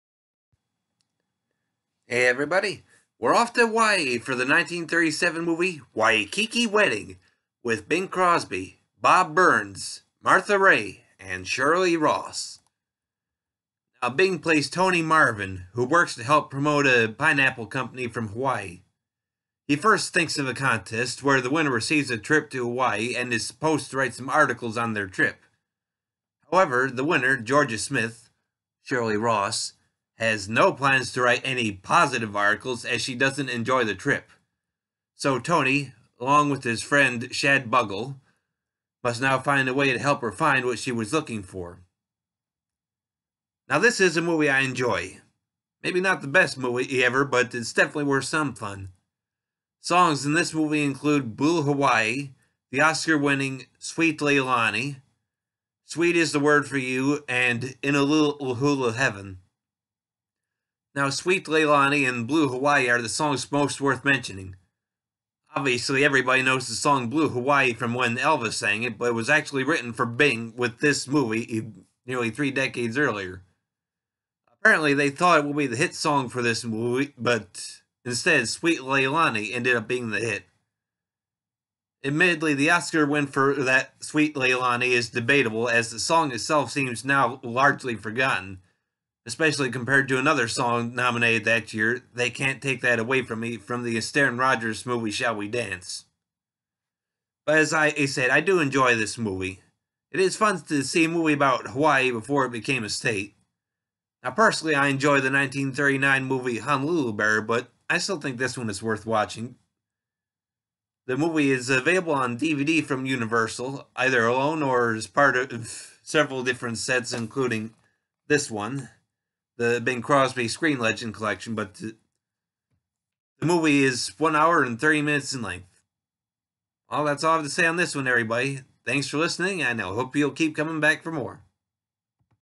Thoughts From The Music(al) Man 2018 on… Waikiki Wedding (1937)
Waikiki Wedding (1937) starring Bing Crosby, Bob Burns, Martha Raye, Shirley Ross
Musical